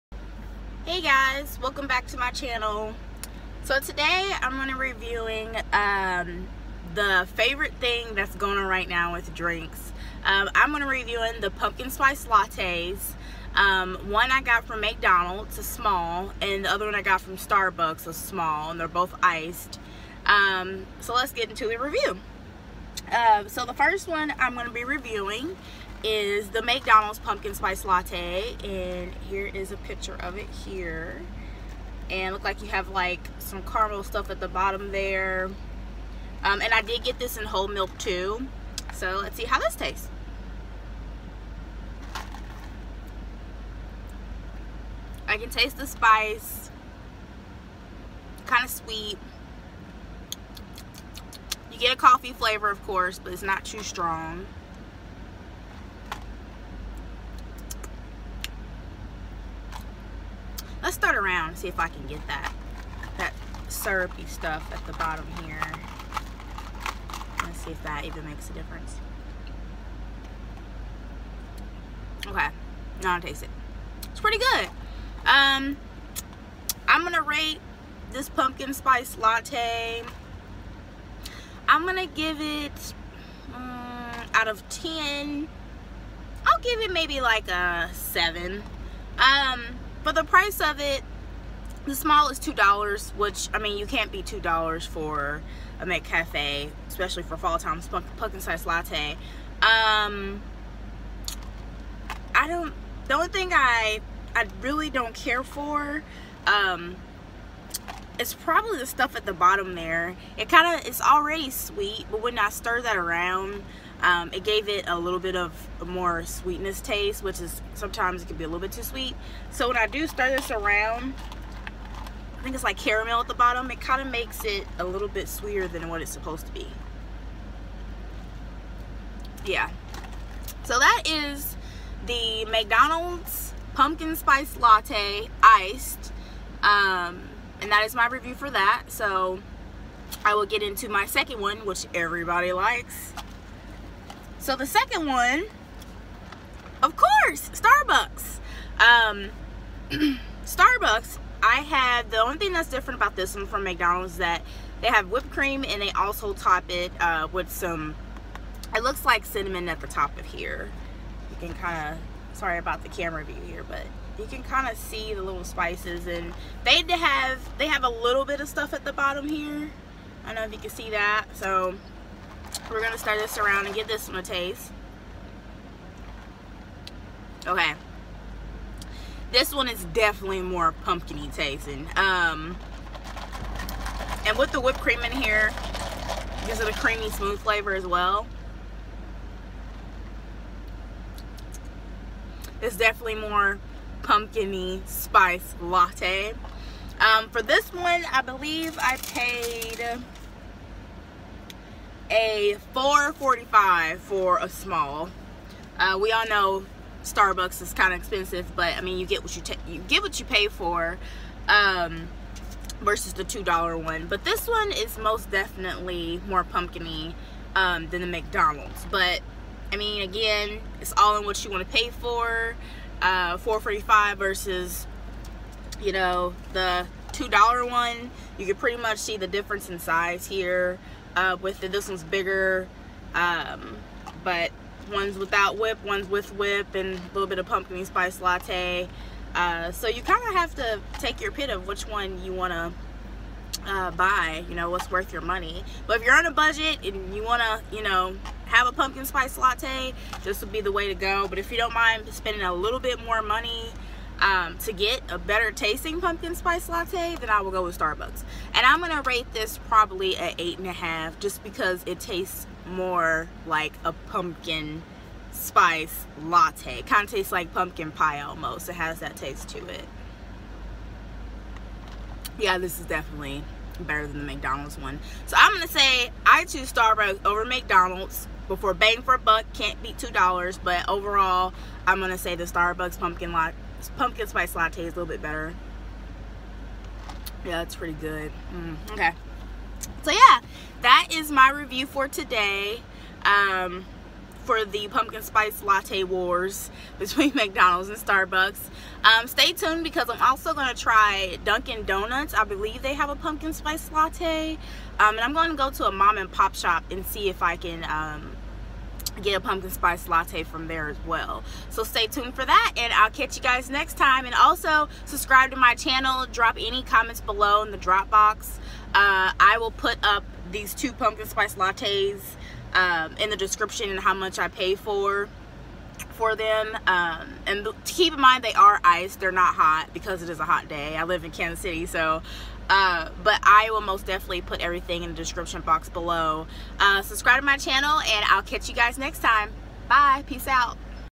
Pumpkin Spice Latte- Starbucks vs. McDonald's
Comparing pumpkin spice lattes...

Starbucks Pumpkin Spice Latte small $4.45 tax not included..

McDonald's Pumpkin Spice Latte small $2.00 tax not included...

Drop comments below and let me know which one is your favorite and why!